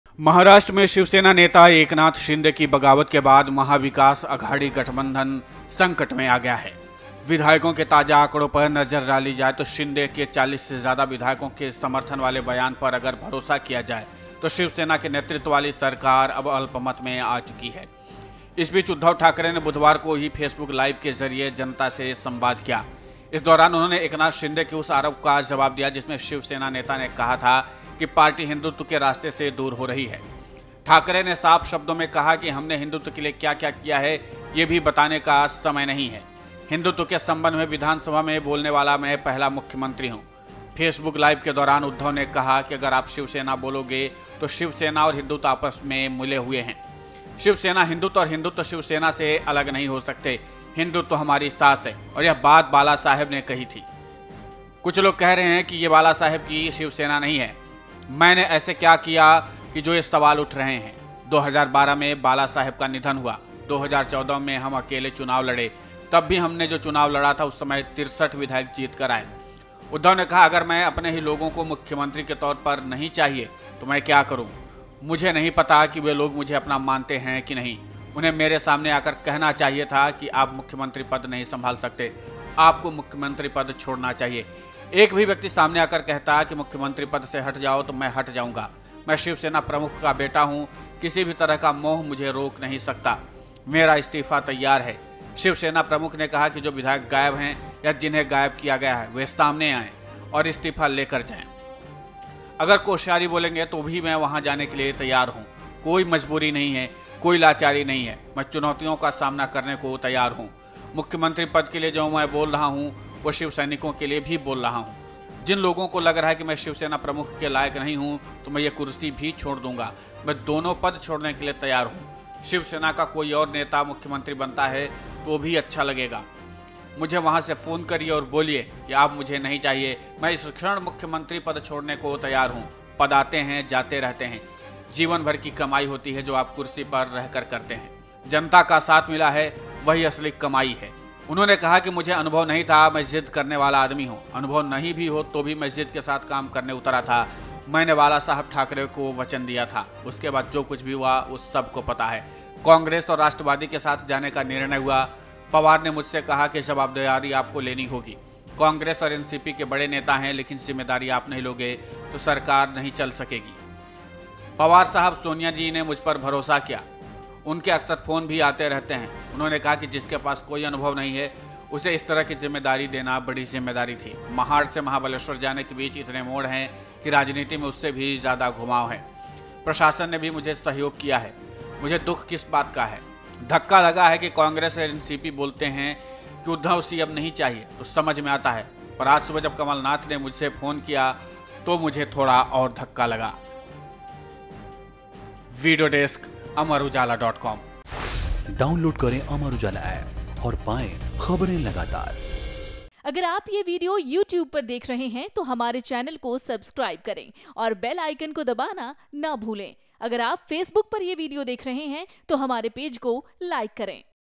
Maharashtra Crisis: बागी नेताओं को सीएम उद्धव का संदेश, उद्धव बोले 'विधायक सामने आकर इस्तीफा मांगें'
Maharashtra Political Crisis: महाराष्ट्र में शिवसेना नेता एकनाथ शिंदे की बगावत के बाद महाविकास अघाड़ी गठबंधन संकट में आ गया है। विधायकों के ताजा आंकड़ों पर नजर डाली जाए और शिंदे के 40 से ज्यादा विधायकों के समर्थन वाले बयान पर भरोसा किया जाए तो शिवसेना के नेतृत्व वाली सरकार अब अल्पमत में आ चुकी है

The Mahavikas Aghadi alliance is in trouble after the revolt of Shiv Sena leader Eknath Shinde in Maharashtra. If the latest figures of MLAs are to be looked at and if Shinde's statement of support of more than 40 MLAs is to be believed, then the Shiv Sena-led government is now in a minority.